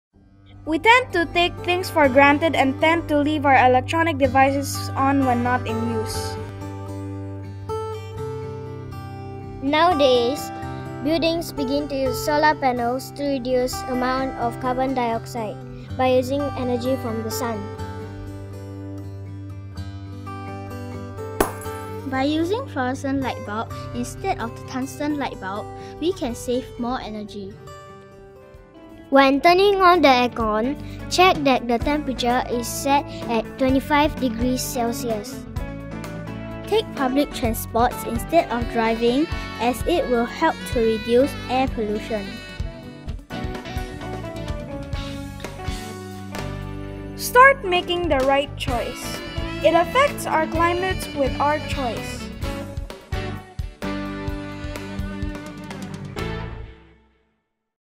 Make the RIGHT Choice
Our life is shaped by the choices we make. Our future will not be determined by chance, but rather by the choices. Start making the RIGHT choice,  it affects OUR climate with our choice!
Created by (FPPS Team 1) Khoo Fu Quan Justin, Seow Xue Qi,Jamie, Lim Tamae and Palmero Jethro Luis Bernal from Farrer Park Primary School for National Climate Change Competition (NCCC) 2015.